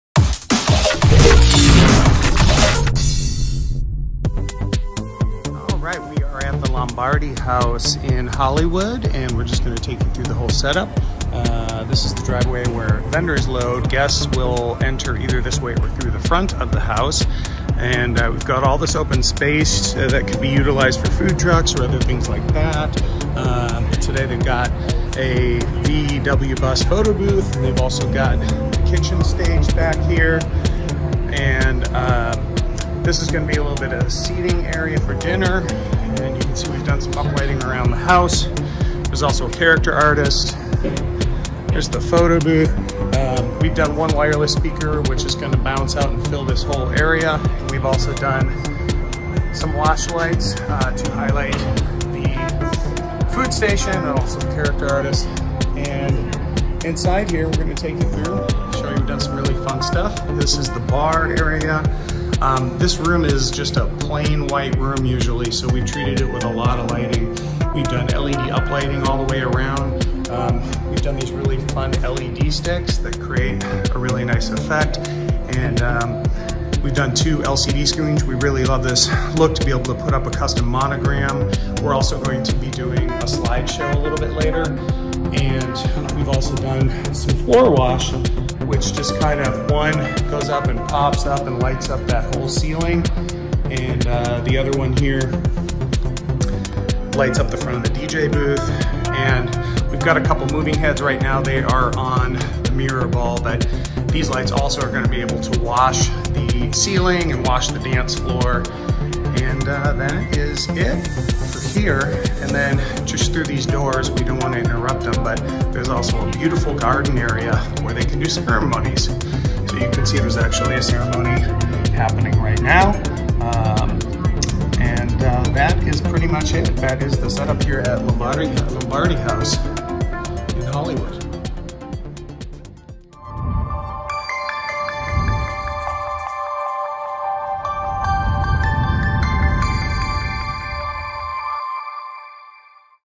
Lombardi House
DJ sound & lighting for event @ Lombardi House Hollywood
Event Design: @sidebysidedesign
Venue: @lombardihouse
DJ / Lighting: @heymisterdjlee
Rentals: @tacer_losangeles
Balloons: @confettiandsprinklesparties
Photographer: @deweythedewster
Sushi: partyandsushicatering
Mocktails: @ilovemrbartender
Photo Booth: @thebusandboothco
Caricatures: @vitaperfetta
Ice Cream: @kingkonela